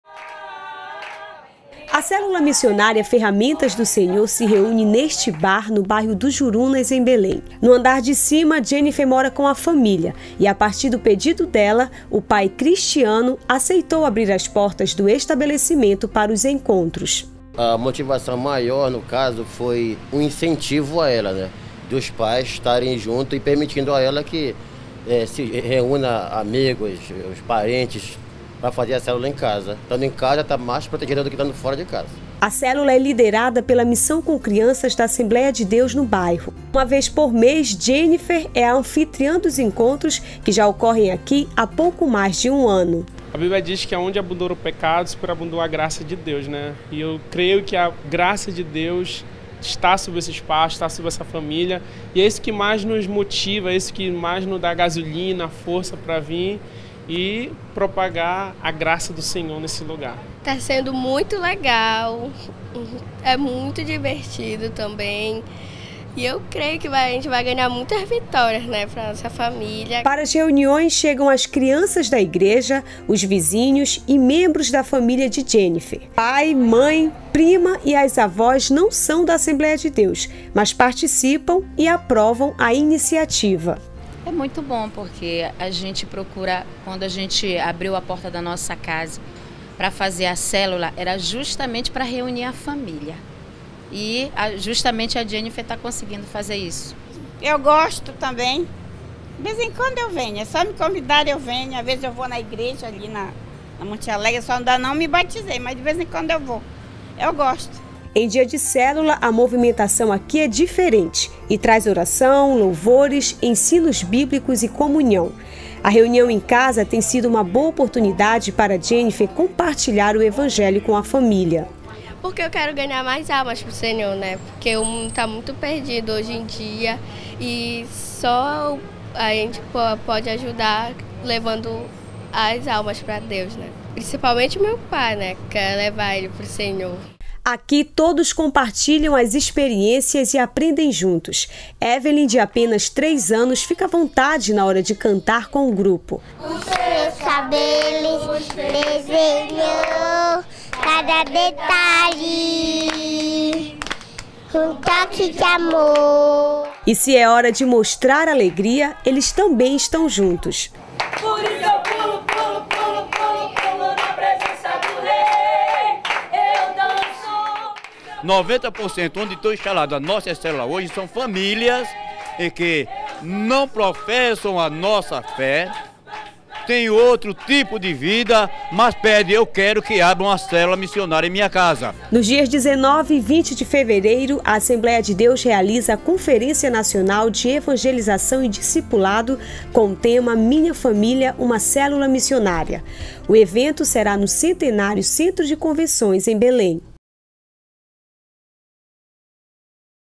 Criança realiza célula missionária em um bar para evangelizar a família.
Jennifer Rosário, de apenas 12 anos, teve a ideia de realizar uma célula missionária para evangelizar os membros da sua família. O curioso é que a célula é feita dentro de um bar no bairro do Jurunas em Belém. A célula existe há mais de um ano e tem feito a diferença na localidade e abençoado a vida desta família. Nos dias 19 e 20 de fevereiro vai ser realizada a Conferência Nacional de Evangelização e Discipulado no Centenário Centro de Convenções em Belém- PA.